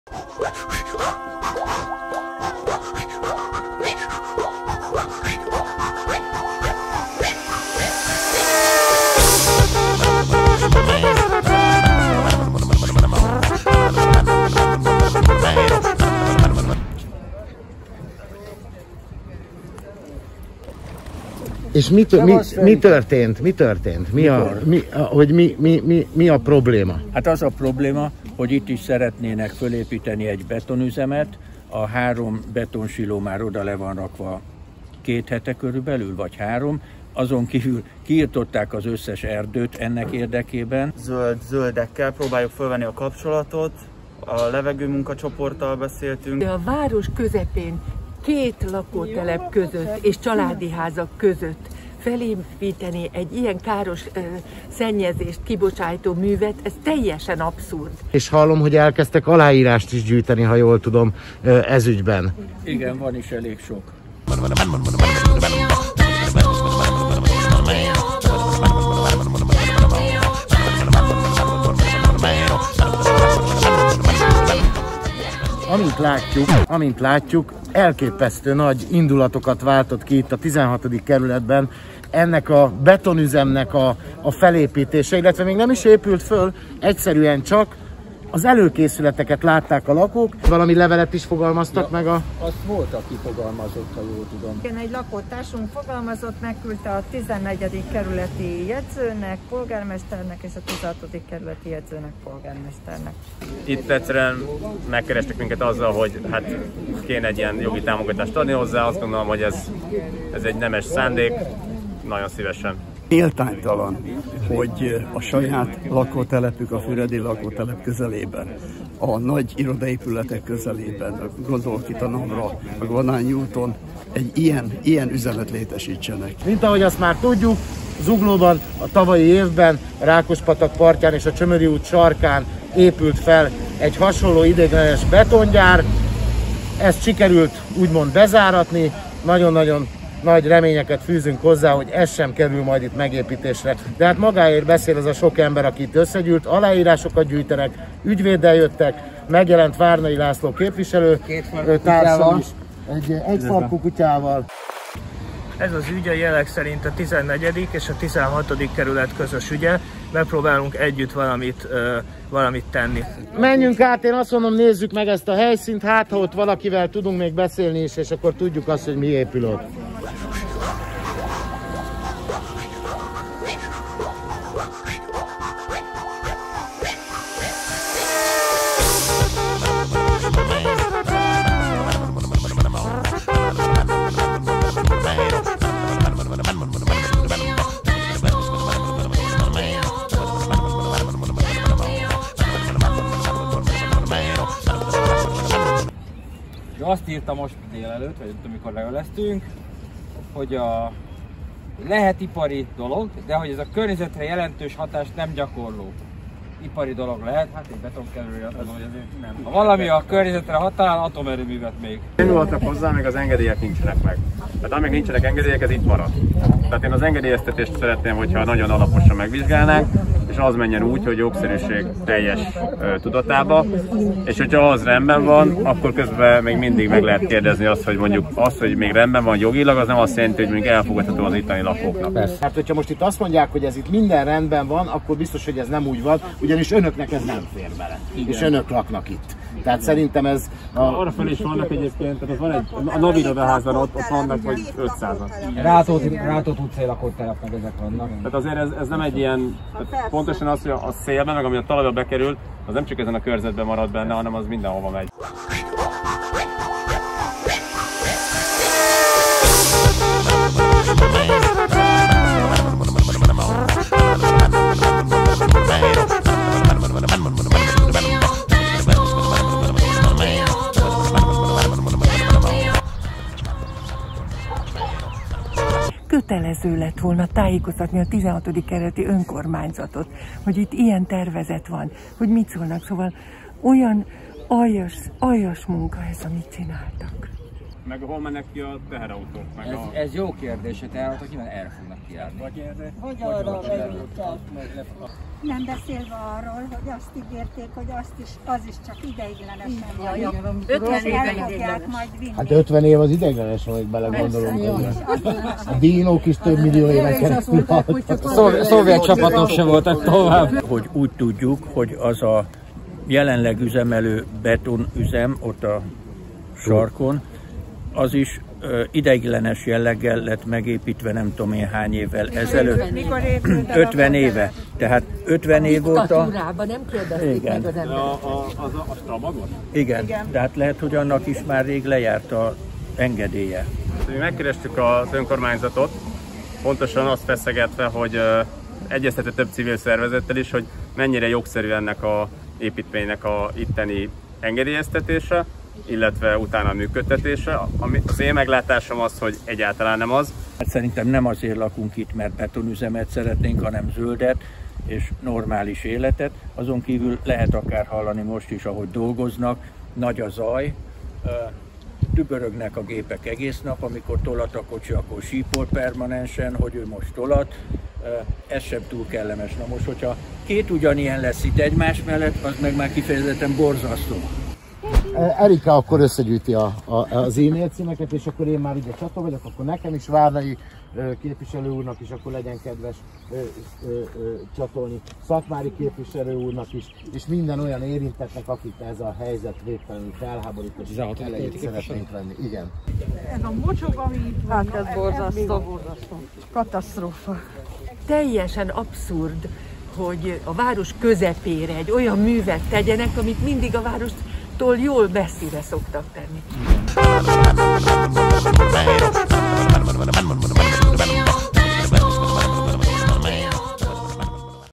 Új betongyár épülhet Zuglóban? -A lakók tiltakoznak
Nem szeretnénk betongyárat Zuglóba! Zugló legyen zöld!

A XIV. kerület Öv utca 17-19 alatti magántelken egy betongyárat szeretne az Uni-City Kft. felépíteni. Az önkormányzat 2020 decemberében adta el ezt a zuglói önkormányzat tulajdonában lévő telket a cégnek. A leendő üzem területe lakóházaktól 80 méterre található. A betongyártáshoz szükséges berendezéseket már leszállították a helyszínre, a talajt már előkészítették, az engedélyek hiányában azonban az építkezést még nem kezdték el.

XVI. kerületi lakók aláírásgyűjtésbe kezdtek, civilek és jogvédők segítségét kérve szeretnék megakadályozni, hogy ez a környezetkárosító üzem felépüljön. Mi, Zuglóiak is szeretnén, ha ez a gyár nem épülne meg. A történet érdekessége, hogy az Öv utcában, alig 100 méterre a tervezett beruházástól, már 50éve működik egy szó szerint nagy port felkavaró giga betongyár,állítólag ideiglenes engedéllyel.

Amennyiben a Füredi úti lakótelepen vagy a környéken laksz és közvetlenül éríntve leszel a környezetszennyező beruházás miatt vagy ha csak egyszerűen Te is abszurdnak tartod, hogy Zuglóban betongyár épüljön, akkor kérlek csatlakozz a tiltakozókhoz, írd alá petíciónkat!

ZUGLÓ LEGYEN TÉNYLEG ZÖLD!